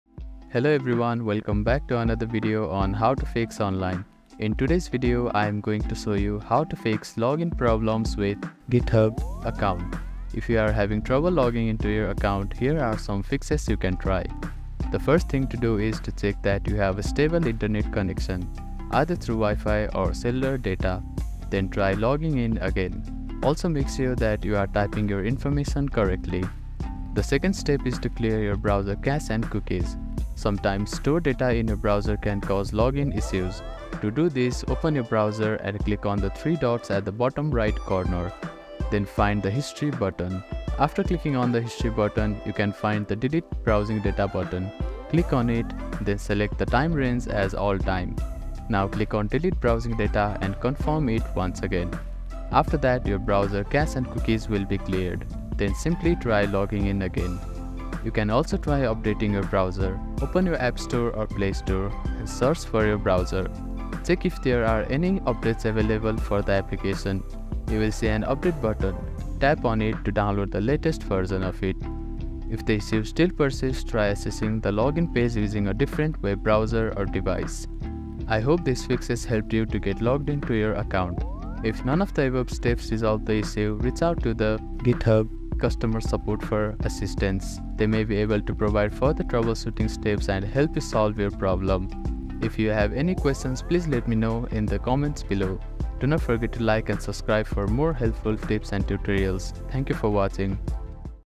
How to Fix Github Account Login Not Working 2024?
In this video, I'll help you troubleshoot and fix issues with logging into your GitHub account. Whether you're facing problems with incorrect credentials, two-factor authentication, or other common login errors, this step-by-step guide will walk you through the solutions to get you back into your account.

By the end of this video, you'll know how to resolve the most common GitHub login issues, ensuring you can access your repositories and projects without hassle.